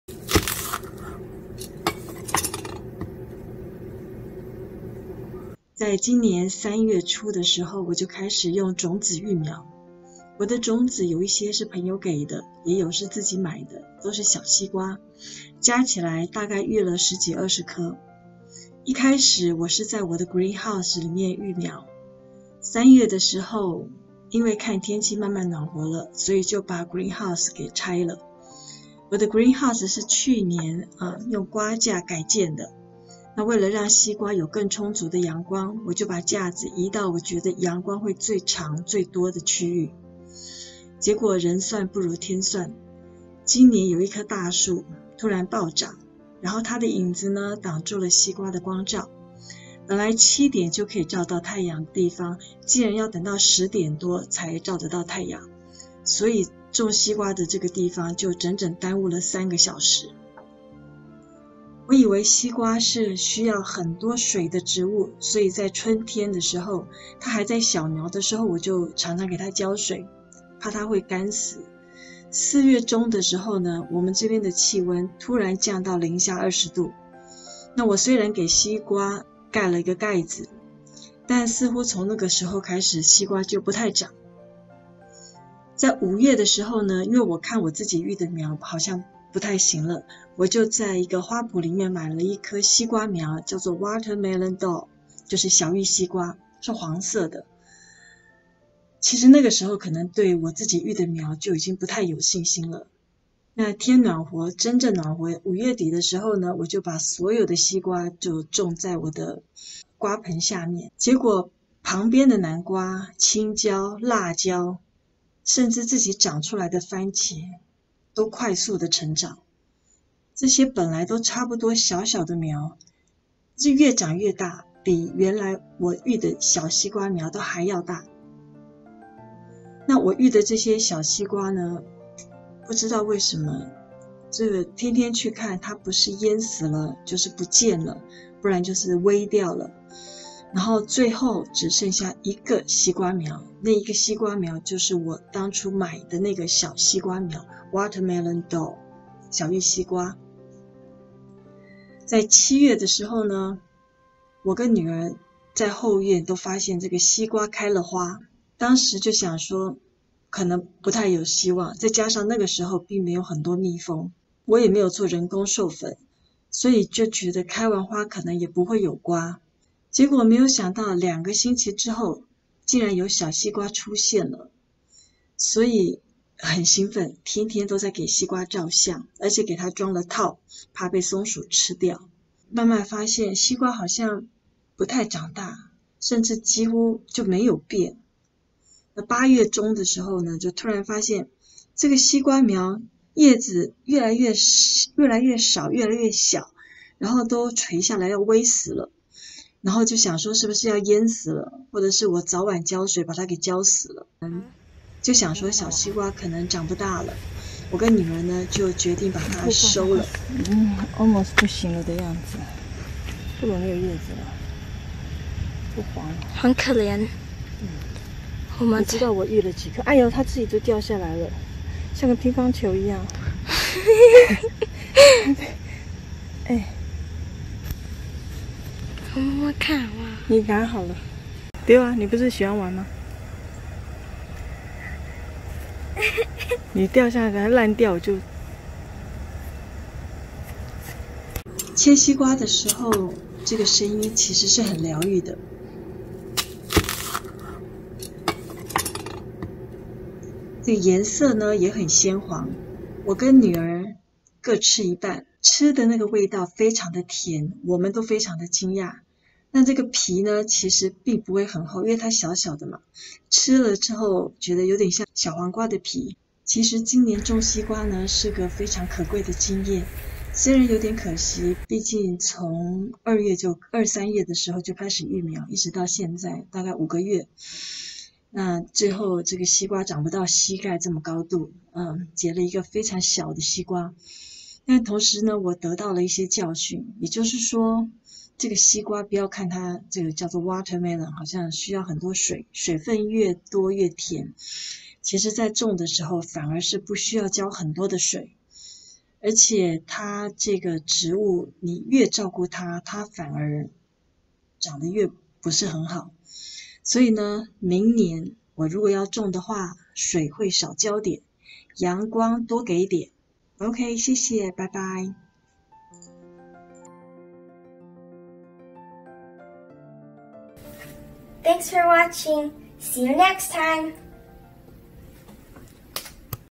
新手種菜2020 種出世界上最小的乒乓球黃西瓜？ world's smallest watermelon doll【歷時5個月紀錄西瓜種植全過程】切開超小超甜的迷你黃西瓜！後院菜地的寶貴經驗全紀錄01
新手種菜2020 種出世界上最小的乒乓球黃西瓜？ world's smallest watermelon doll【歷時5個月紀錄西瓜種植全過程】切開超小超甜的迷你黃西瓜！後院菜地的寶貴經驗全紀錄！
種西瓜是我今年後院菜地的新挑戰，「watermelon doll」，「suger baby watermelon」，「crimson sweet watermelon」都是非常推薦種植的小西瓜品種，因為我很懷念台灣又黃又甜的小玉西瓜，所以初春防疫期間就開始用種子育苗，總共育了將近20顆，最後八月時採收西瓜，是唯一僅存活的一棵苗，也是僅有的唯一西瓜！影片介紹了這五個月來的風風雨雨，學到經驗和教訓，也吃到了世界上最小的乒乓球黃西瓜！
00:00 開始
00:06 西瓜種子開始育苗
00:18 簡易小暖棚 Green House
00:32 瓜架換位
00:40 錯誤經驗: 光照問題
01:00 錯誤經驗: 澆水問題
01:12 錯誤經驗: 氣溫問題
01:25 買小玉西瓜苗 Watermelon Doll
01:45 所有西瓜苗移植到後院菜園
02:30 西瓜苗開花
02:52 小西瓜出現
03:24 採收小玉西瓜
04:01 切西瓜
04:15 吃西瓜感想
04:26 第一次種植西瓜得到的教訓和感想

這裡的影片是我來美國後的生活點滴，包括我的日常，料理、種菜、居家生活、休閒興趣分享！